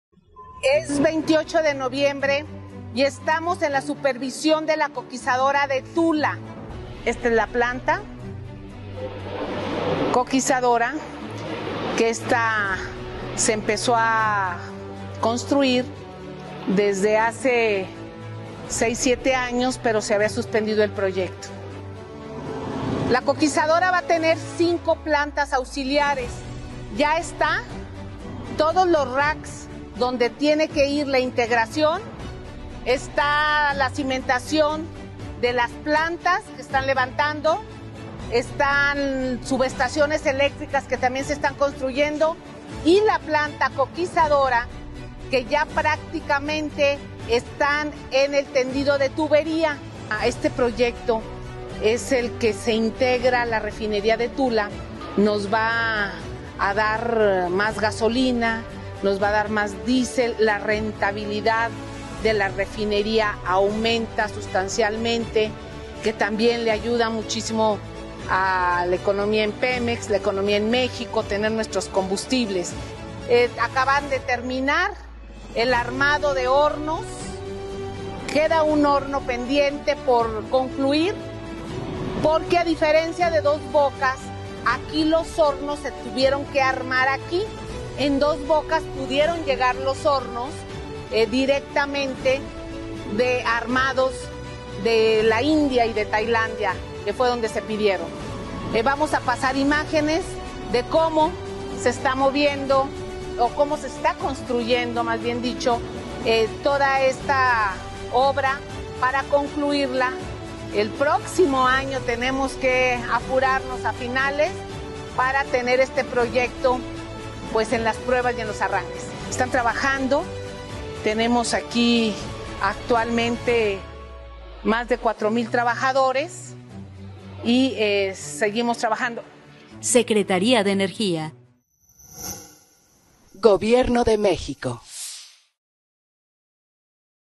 Más de 4 mil trabajadores construyen la planta Coquizadora de Tula
El presidente  López Obrador ha reportado sobre el proyecto de la planta de coquización en la refinería de Tula,Hidalgo de ⁦Pemex⁩.

Con esto vamos a producir mayor volumen de combustibles bajo en azufre y eliminar el combustoleo.